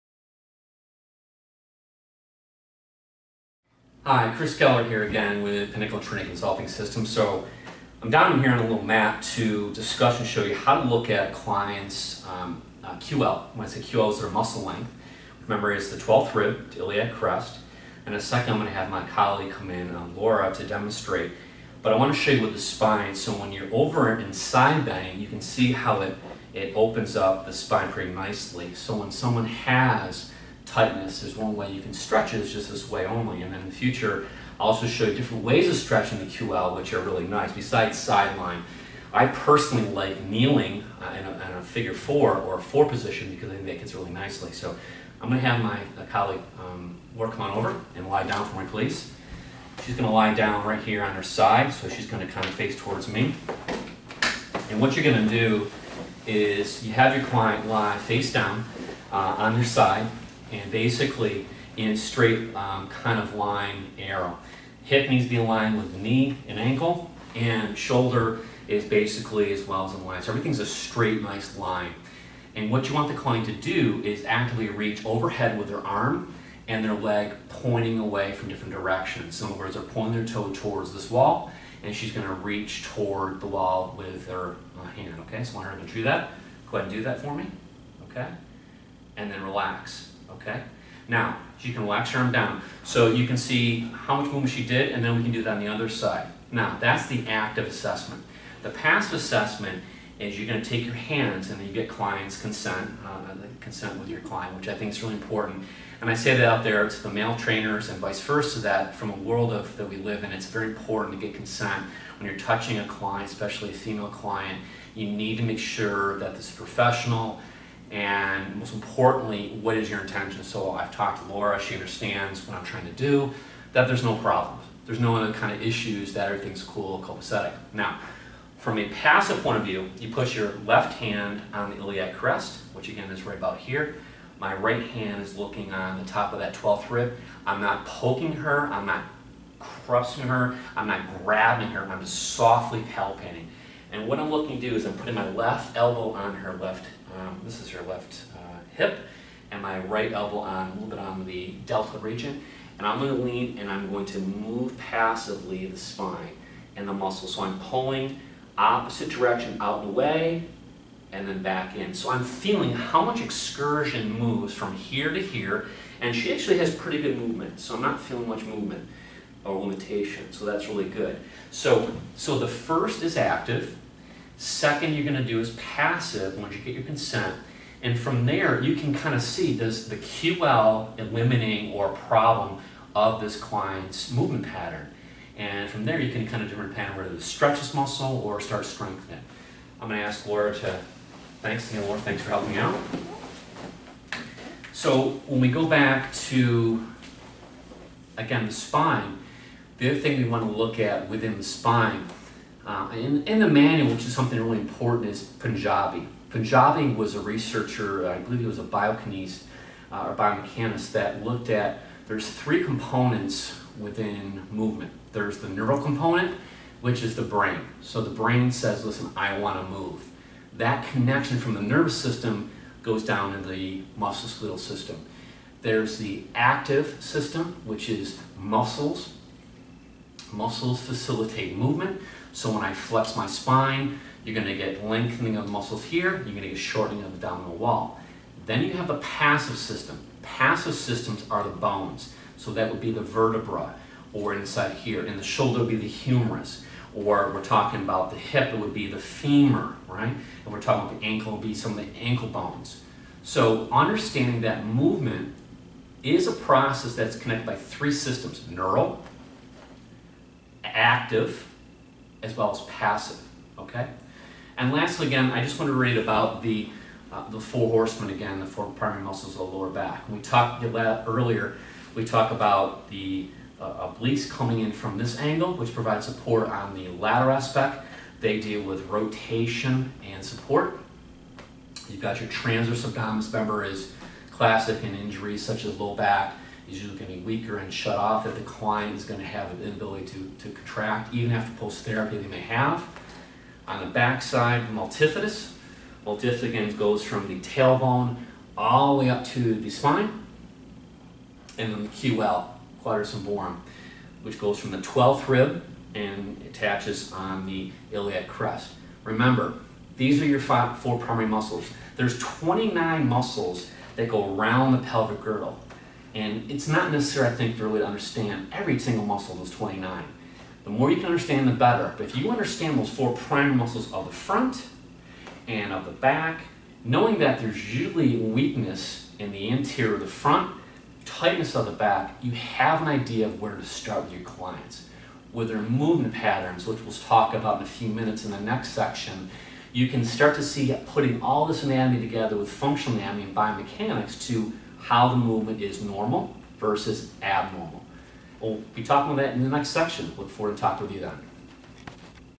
Video 5 How to assess the QL and why
The quadratus lumborum(QL), a dynamic supporting muscle of the pelvic girdle that commonly tightens with low back injuries. The research shows that if the QL continues to tighten during injury, can refer pain to the inner or outer thigh(Travell & Simmons). Learn more about what the role of the QL is, how to assess, stretch and keep happy.